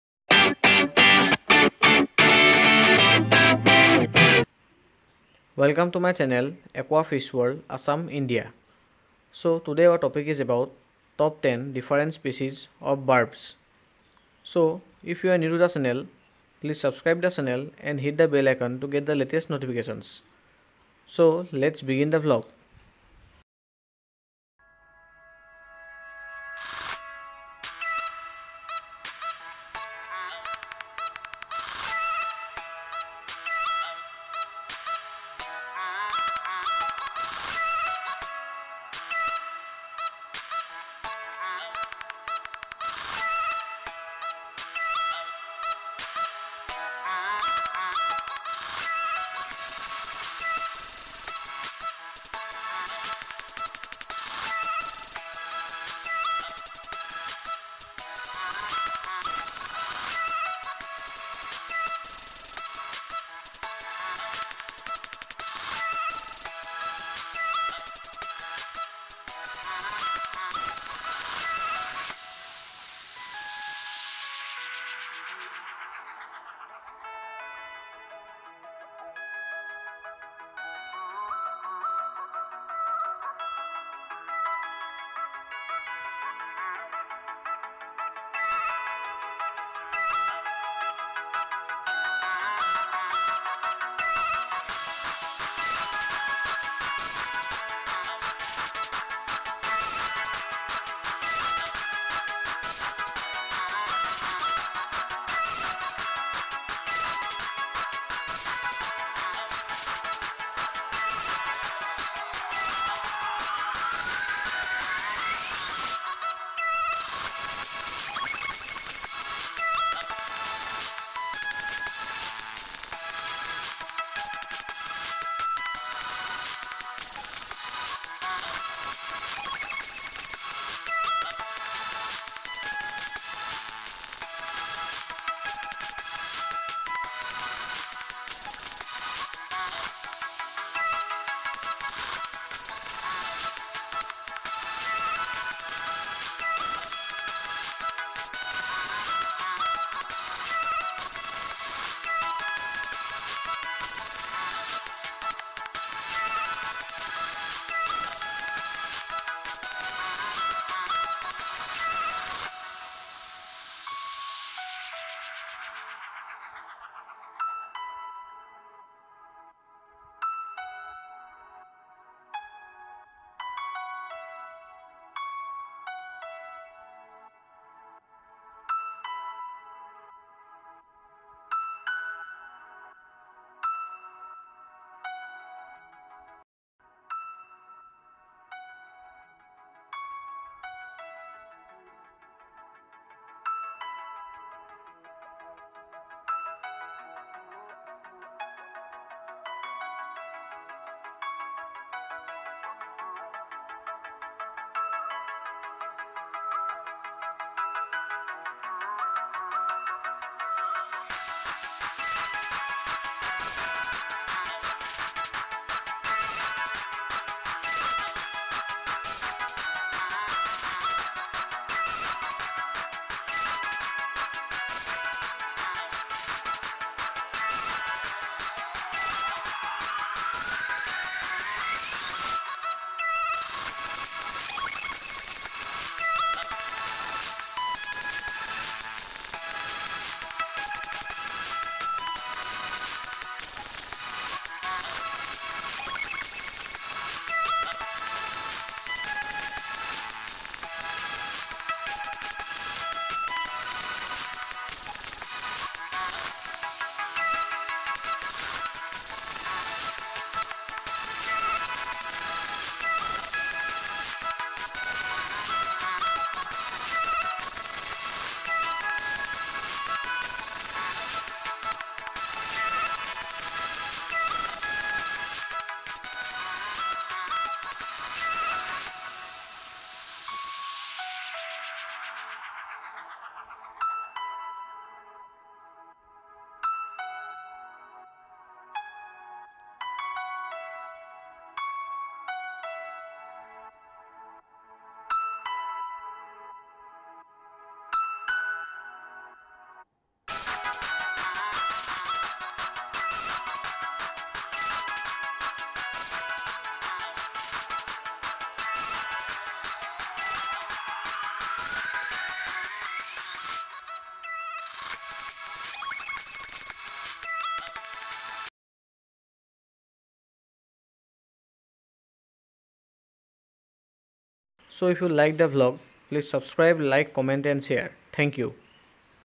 Top 10 different species of barbs... 🐟🐟
Today's vlog is about different species of barbs in the world.